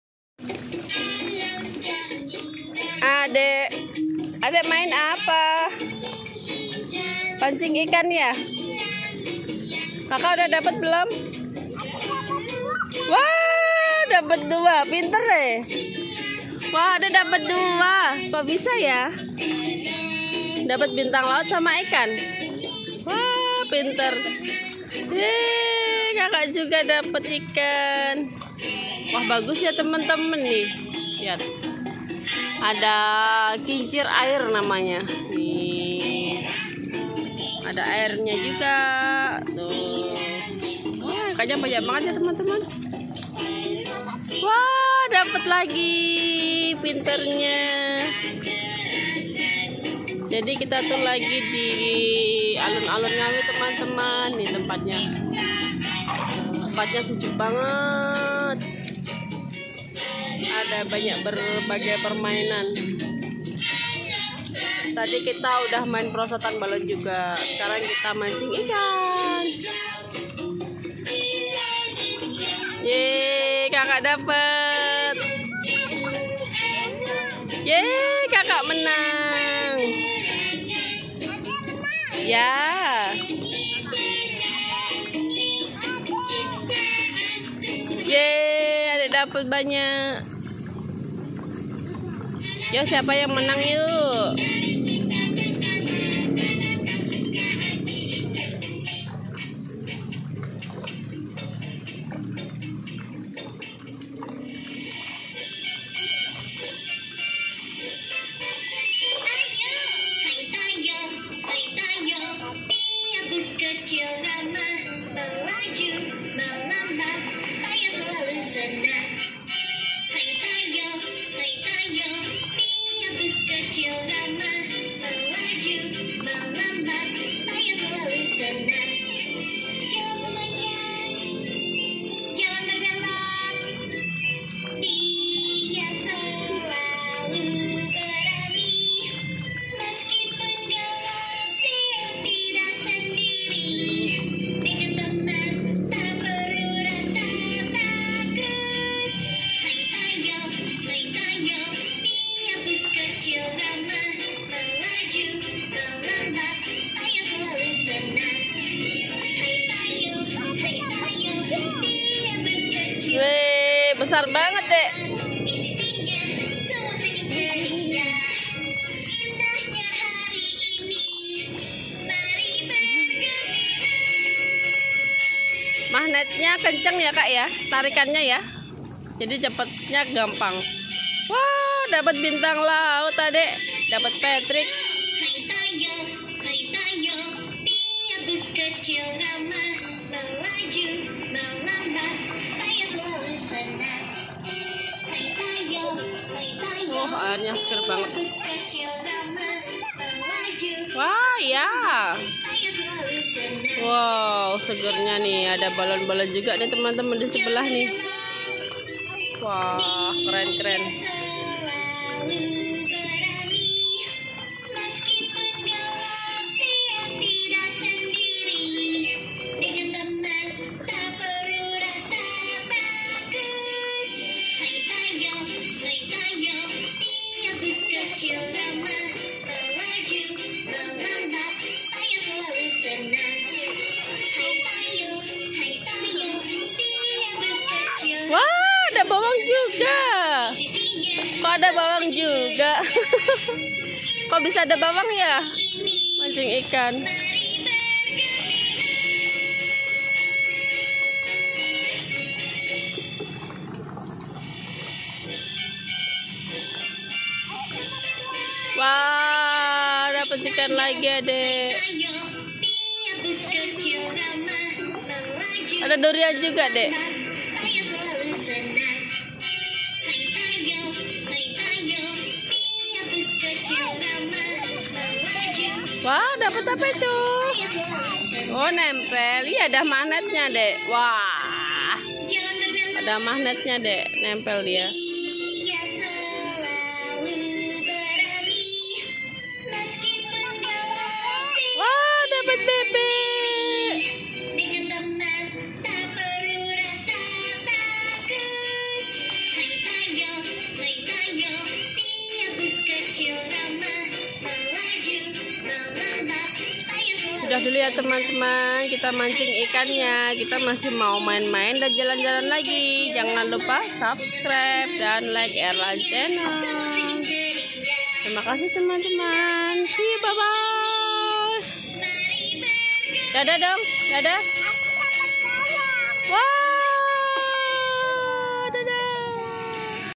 Bermain pancing ikan di Alun Alun Ngawi
Halo teman teman kali ini Erland dan Nabila min pancing ikan di Alun alun kota Ngawi, kota ngawi adalah kota kelahiran mama dan papaku. Seru banget saat kita mudik lebaran kemarin kita sempatkan main di sini

main pancing ikan
air mancur
alun alun ngawi
bermain di alun alun
pancing ikan
dunia anak anak
bermain bersama
erland dan nabila
erland channel